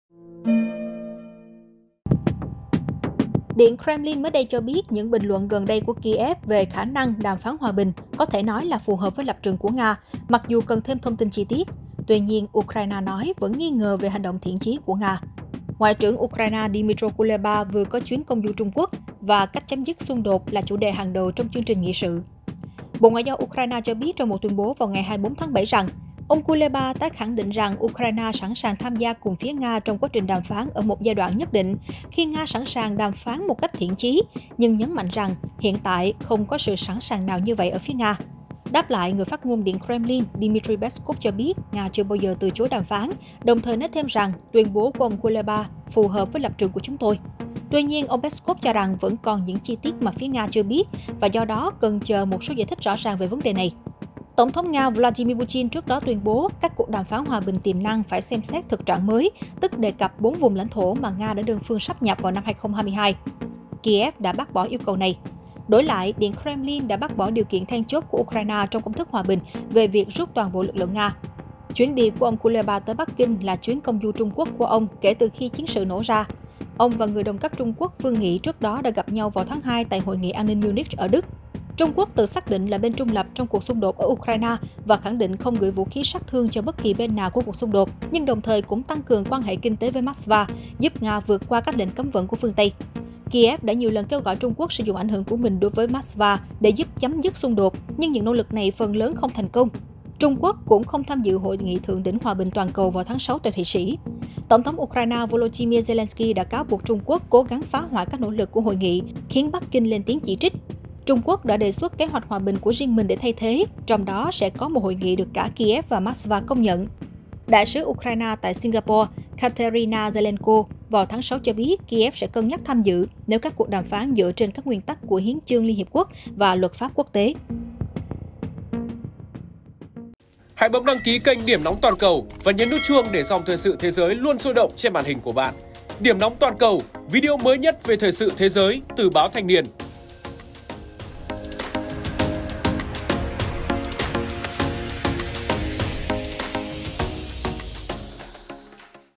Điện Kremlin nói Ukraine, Nga "đồng điệu" hơn về đàm phán; Kyiv hoài nghi
Điện Kremlin cho biết những bình luận gần đây của Kyiv về khả năng đàm phán hòa bình “có thể nói là phù hợp với lập trường của Nga” mặc dù cần thêm thông tin chi tiết. Tuy nhiên Ukraine nói vẫn nghi ngờ về “hành động thiện chí” của Nga.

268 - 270 Nguyễn Đình Chiểu, Phường Võ Thị Sáu, quận 3, TP HCM